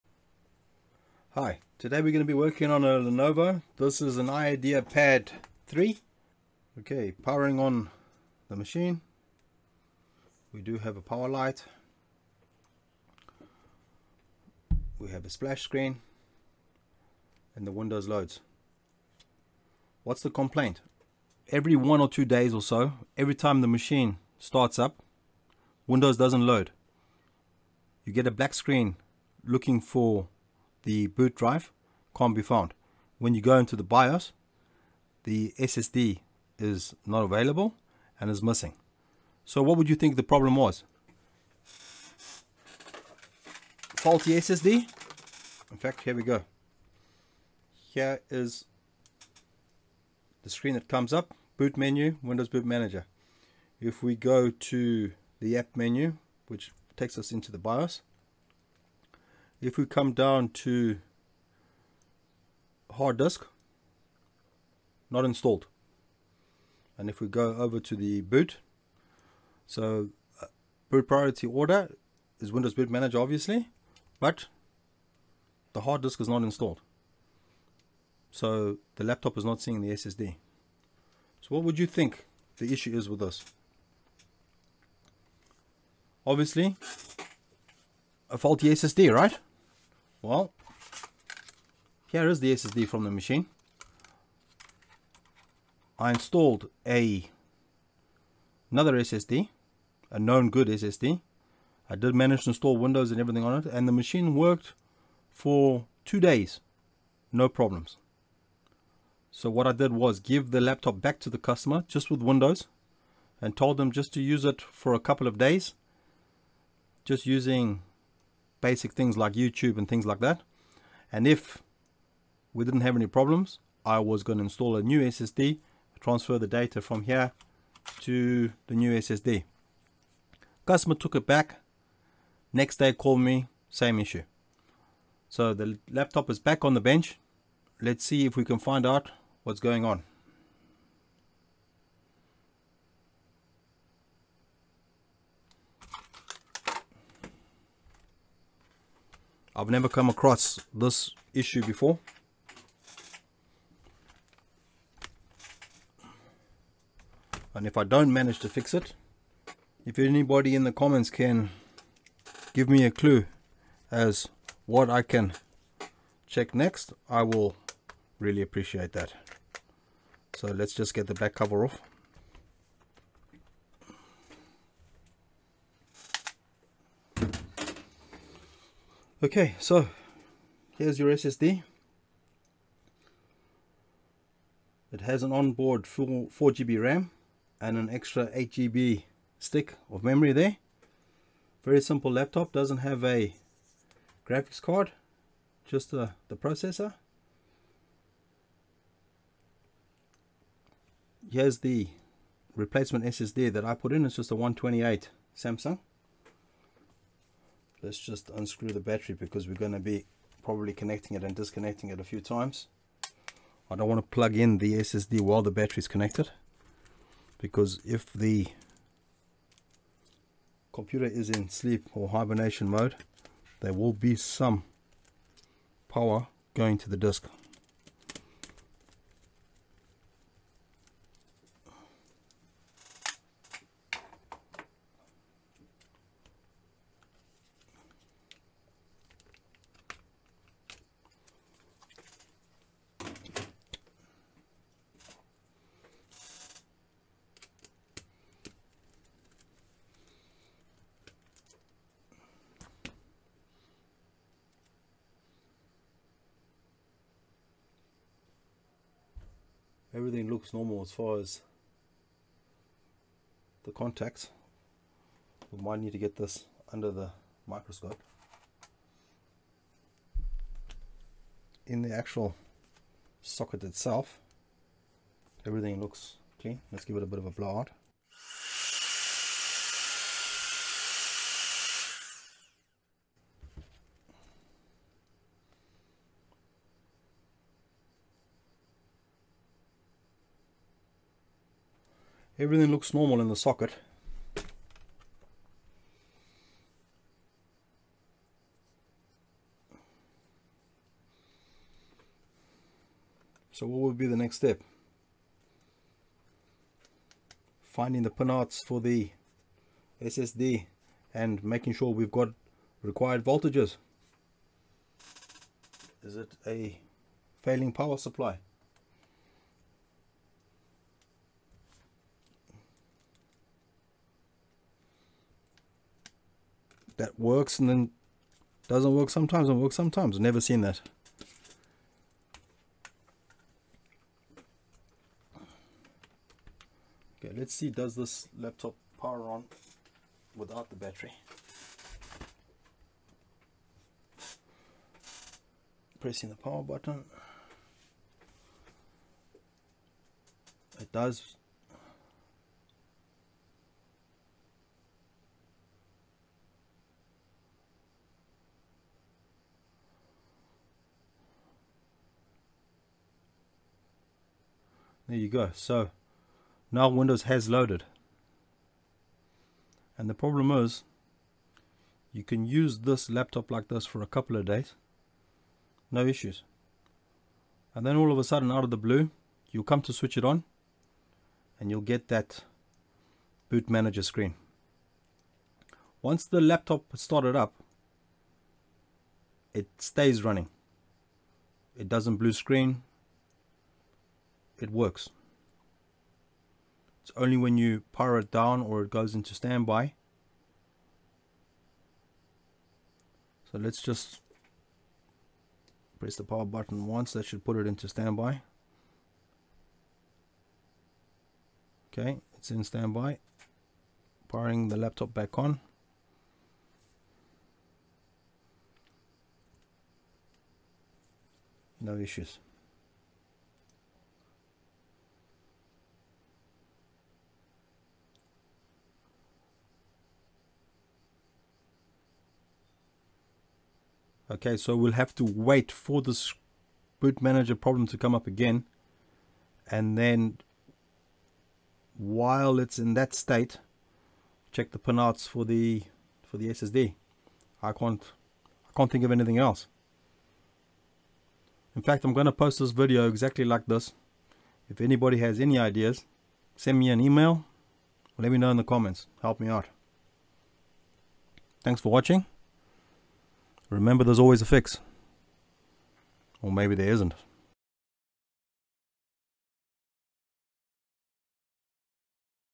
Lenovo Ideapad losing m2 SSD at startup randomly
Lenovo laptop sometimes starts normally but randomly fails to boot, windows cannot find a boot drive and the m.2 SSD is not showing in the bios. If the laptop does boot it stays and runs all day no issues. SSD replaced and problem still exists. Bios updated to latest version didn't help. Is the 3v power supply dropping out? or is it bad contacts or maybe it will come down to data logic signals.